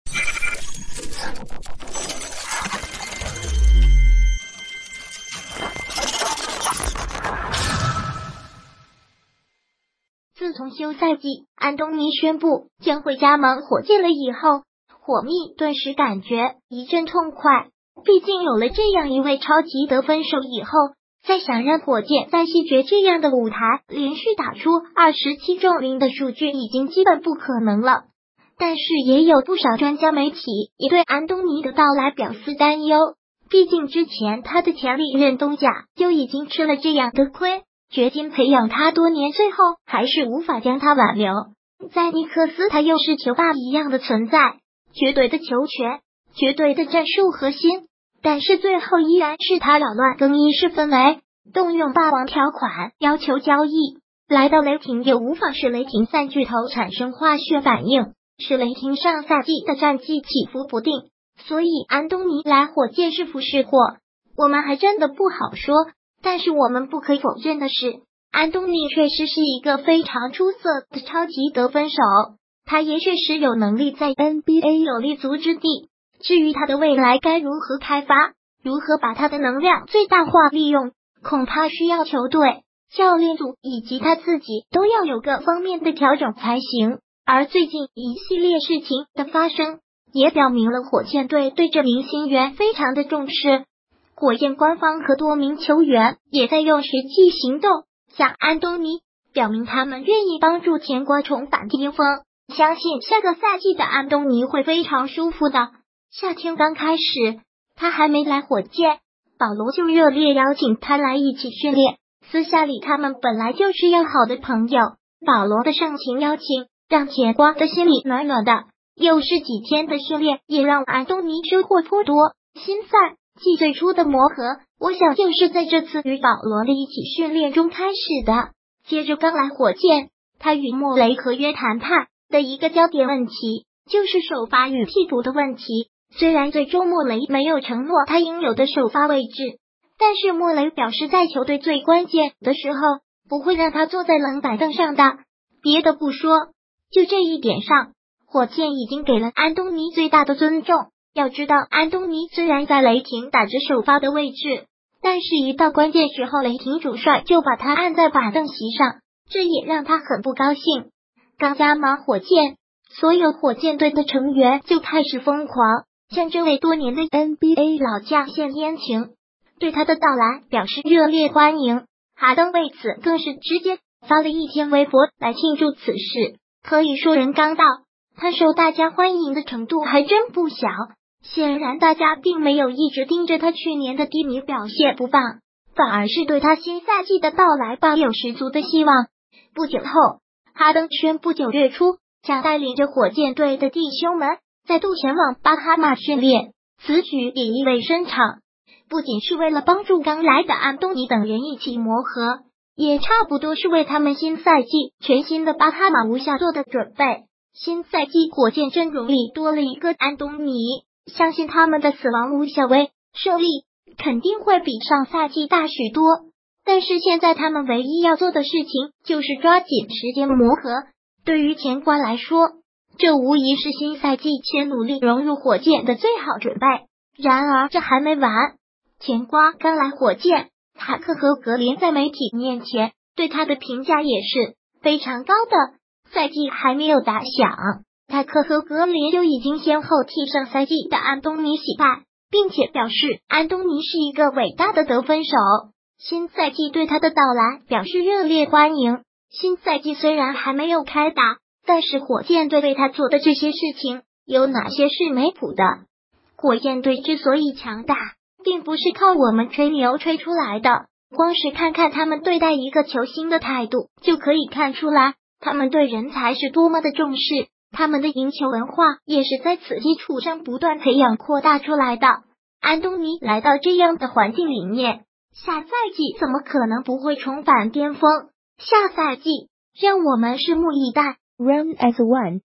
安东尼真舒服，火箭多名大将愿助他重返巅峰，新赛季火箭欲起飞！
自从休赛季安东尼宣布将会加盟火箭了以后，火蜜顿时感觉一阵痛快，毕竟有了这样一位超级得分手以后，再想让火箭在西决这样的舞台连续打出27中0的数据已经基本不可能了！但是也有不少专家媒体也对安东尼的到来表示担忧，毕竟之前他的潜力任东家就已经吃了这样的亏，掘金培养他多年最后还是无法将他挽留，在尼克斯他又是球霸一样的存在，绝对的球权，绝对的战术核心，但是最后依然是他扰乱更衣室氛围，动用霸王条款要求交易！来到雷霆又无法使雷霆三巨头产生化学反应，使雷霆上赛季的战绩起伏不定！所以安东尼来火箭是福是祸，我们还真的不好说！但是我们不可否认的是，安东尼确实是一个非常出色的超级得分手，他也确实有能力在nba有立足之地！至于他的未来该如何开发，如何把他的能量最大化利用，恐怕需要球队，教练组以及他自己都要有各方面的调整才行！而最近一系列事情的发生也表明了火箭队对这名新援非常的重视，火箭官方和多名球员也在用实际行动向安东尼表明他们愿意帮助甜瓜重返巅峰，相信下个赛季的安东尼会非常舒服的！夏天刚开始，他还没来火箭，保罗就热烈邀请他来一起训练，私下里他们本来就是要好的朋友，保罗的盛情邀请让甜瓜的心里暖暖的，又是几天的训练